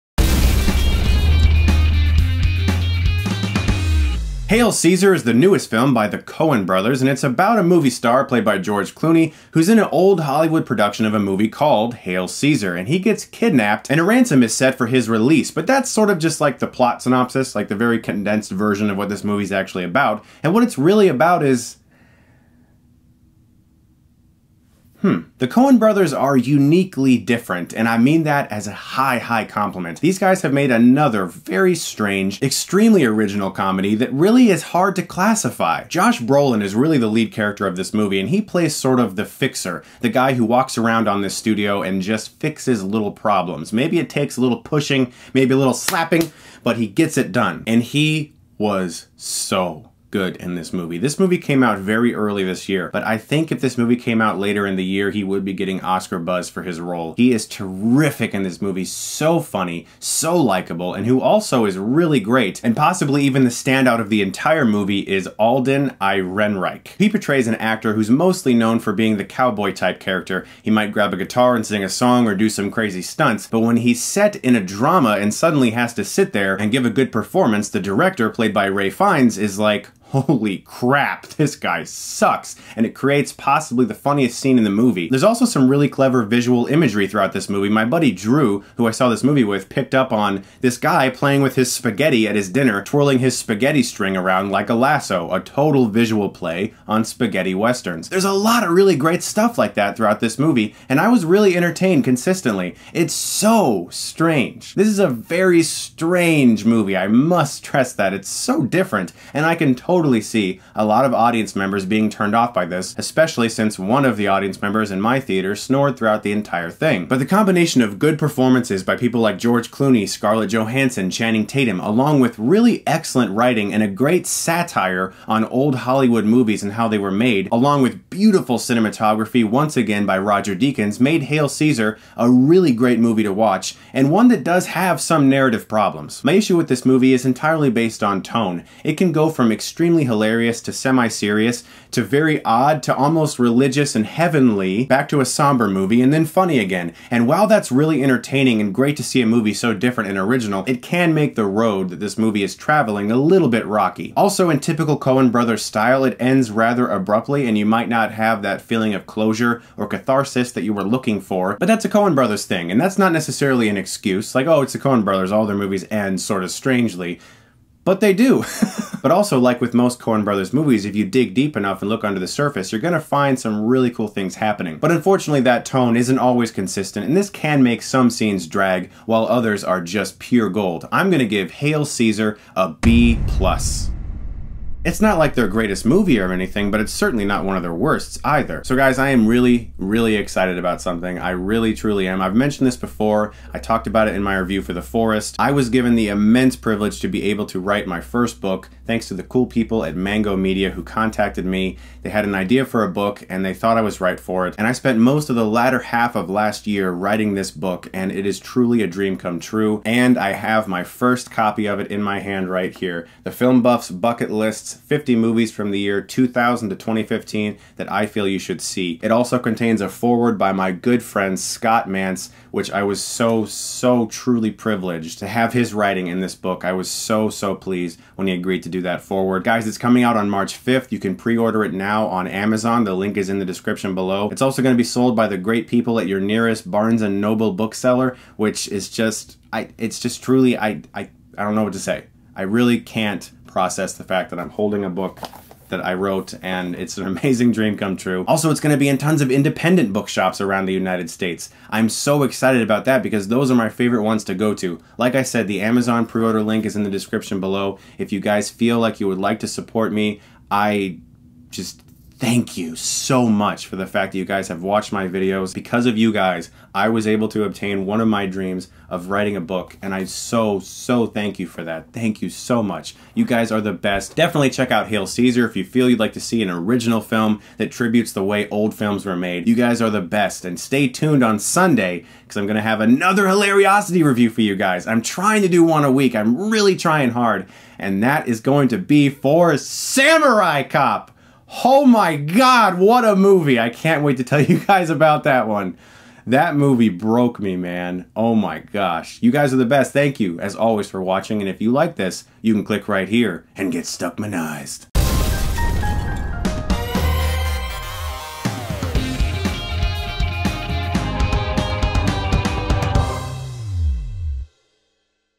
Hail, Caesar! - Movie Review
You can preorder my book from AMAZON

Chris Stuckmann reviews Hail, Caesar!, starring Josh Brolin, George Clooney, Alden Ehrenreich, Ralph Fiennes, Jonah Hill, Scarlett Johansson, Frances McDormand, Tilda Swinton, Channing Tatum. Directed by Joel Coen, Ethan Coen.